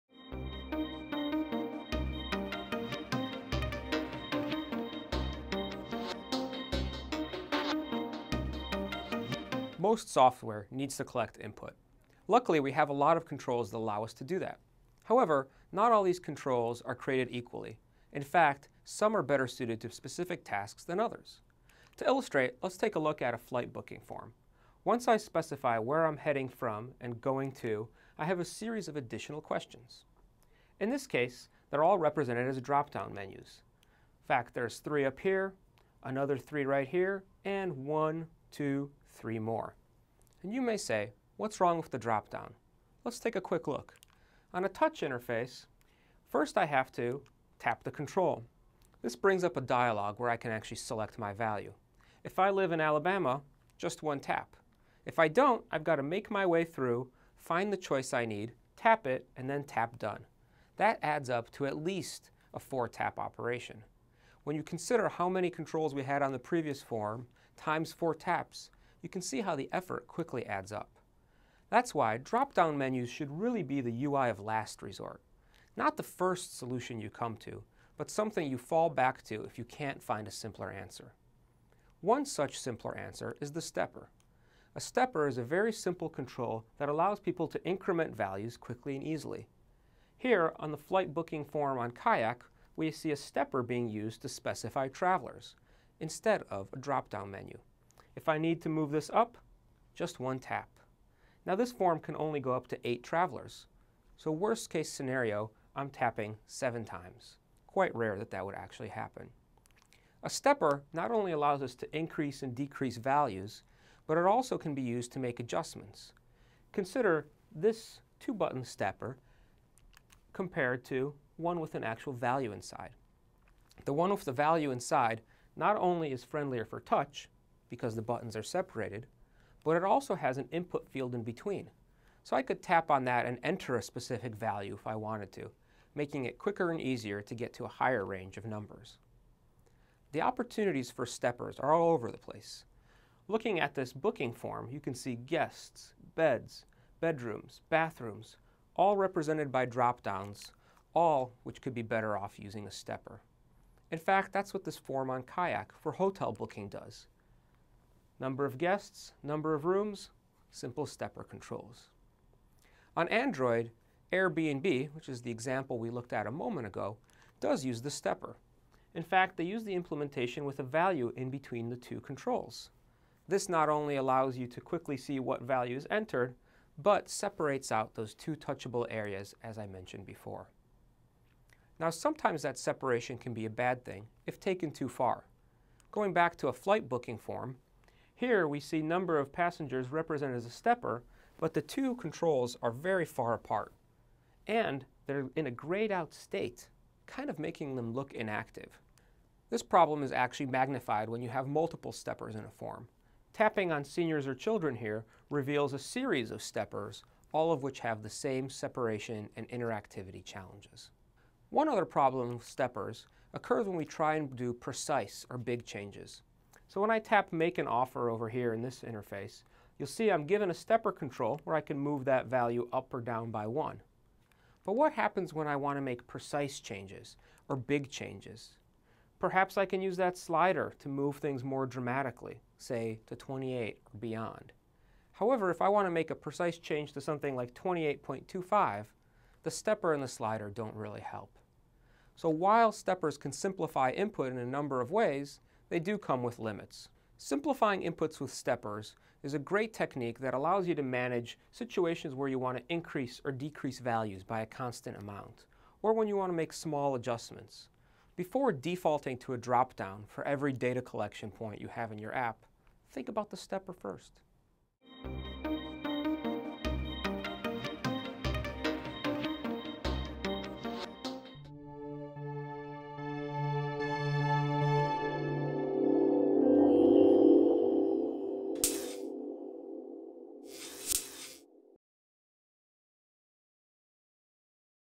How to Simplify Input with Steppers | Intel Software
Once again, Luke W, shines the light of truth on another UX myth! Today, how stepper controls could send the faithful, old dropdown, the way of the dinosaurs.